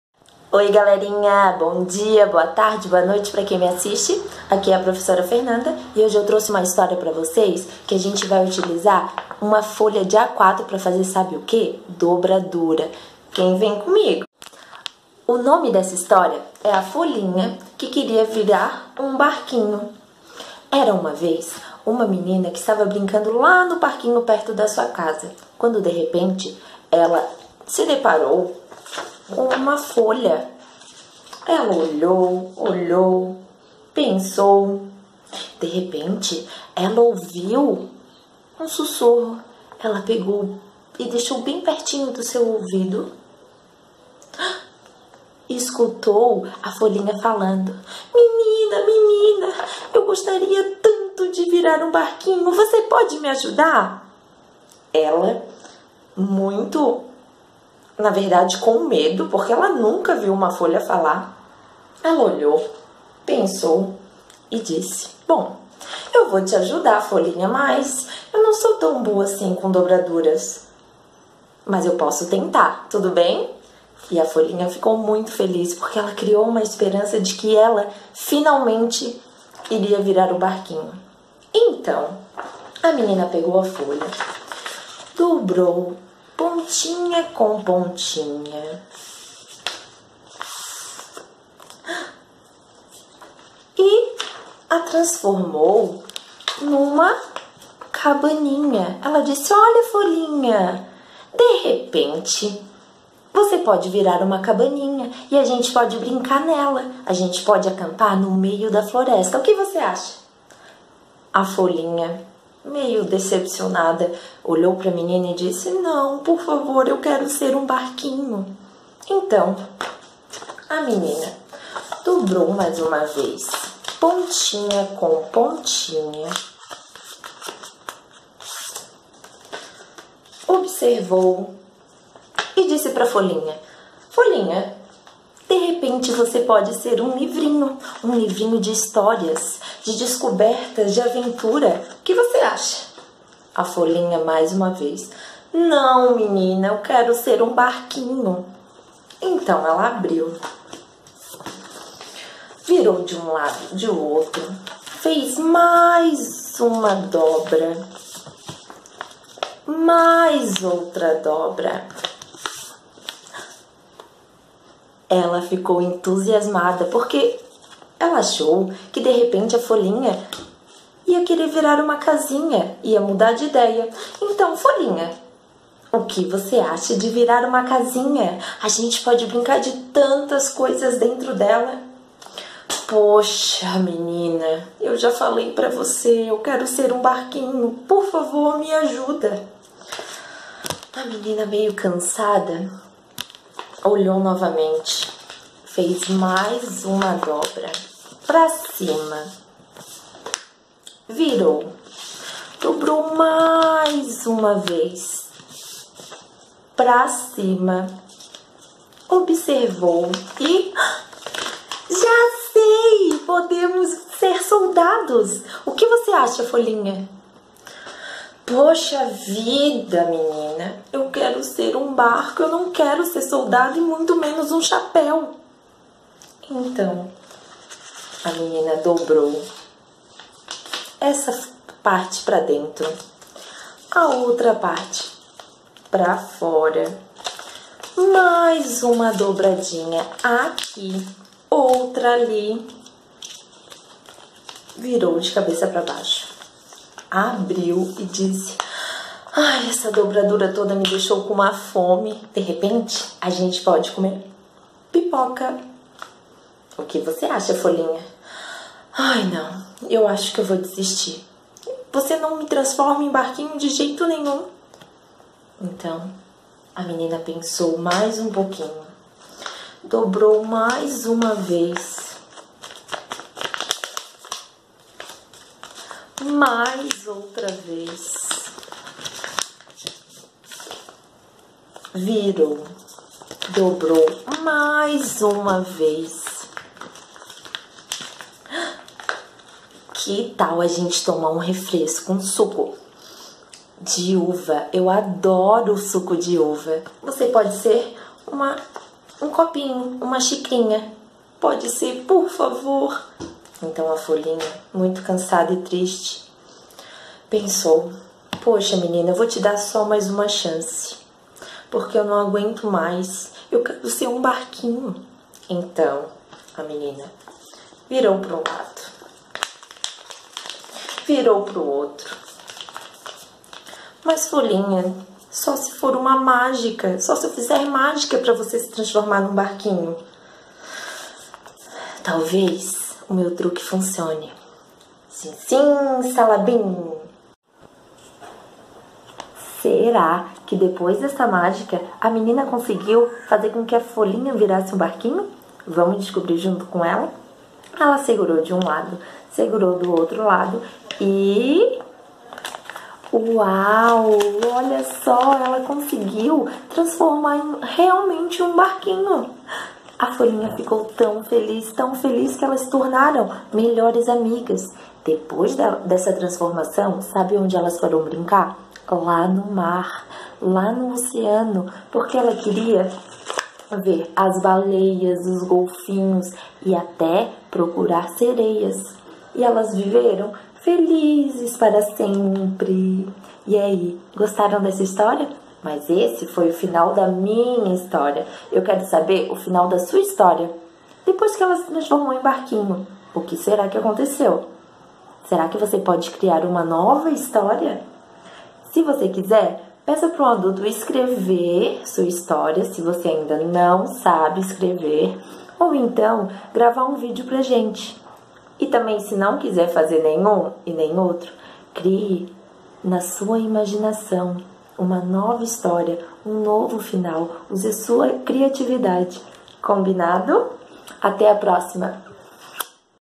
"Era uma vez” História: A folha que queria virar um barquinho
Oi, crianças!Estamos selecionando várias histórias para você se divertir durante esse tempo que você está em casa. E dessa vez teremos a participação da Professora Fernanda! A professora Fernanda preparou uma história bem legal pra gente, chama todo mundo e vem conhecer a História da folha que queria virar um barquinho, além disso, vamos aprender a fazer uma dobradura!